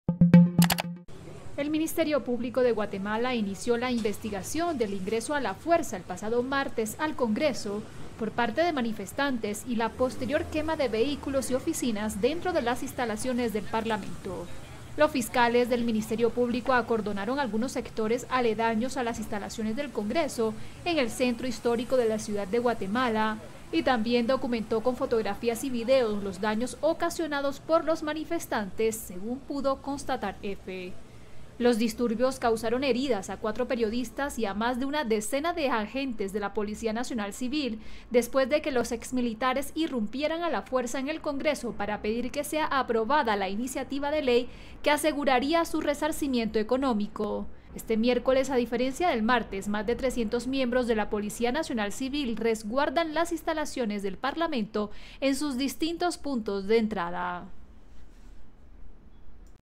Policía de Guatemala resguarda el Congreso mientras Fiscalía investiga ingreso a la fuerza
El Ministerio Público (MP, Fiscalía) de Guatemala investiga este miércoles el ingreso a la fuerza el martes al Congreso por parte de manifestantes y la posterior quema de vehículos y oficinas dentro de las instalaciones del Parlamento.